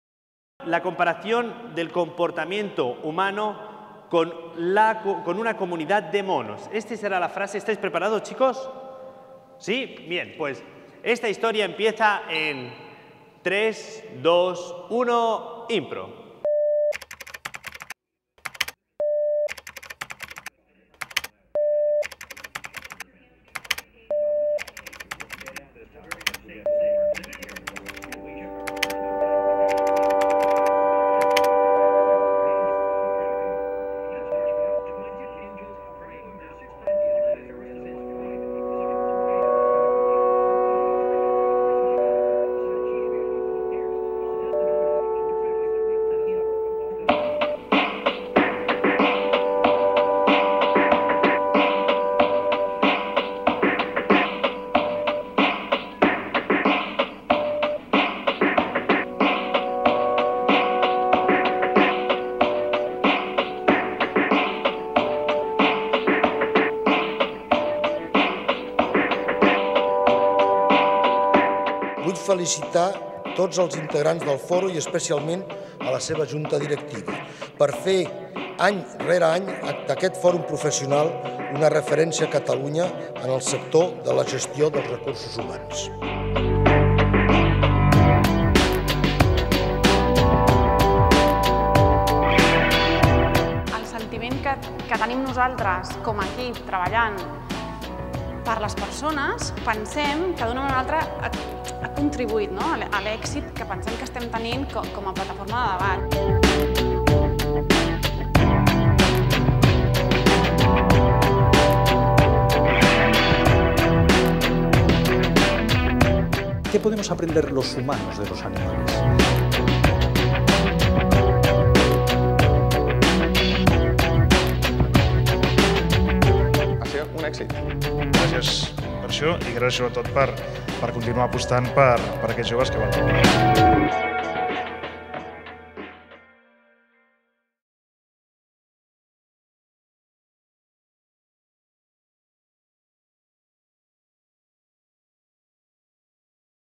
X Encuentro Anual Foro RRHH - Resumen
En el décimo aniversario del Encuentro Anual, el Foro de RRHH sorprendió con las experiencias que más HUELLA HAN DEJADO en los últimos diez años. La sesión, celebrada el pasado el 29 de novembre en el incomparable marco de la Capella del MACBA se pudo disfrutar de ideas novedosas sobre temas como trabajo en equipo o liderazgo, así como participar activamente y revivir los momentos más emocionantes del Foro de RRHH.

Durante esta sesión, contamos la participación del conferenciante y escritor Enrique de Mora, y del grupo Improshow quien realizó una actividad en grupo. El Foro de RRHH està patrocinado por Randstad y Cheque Gourmet.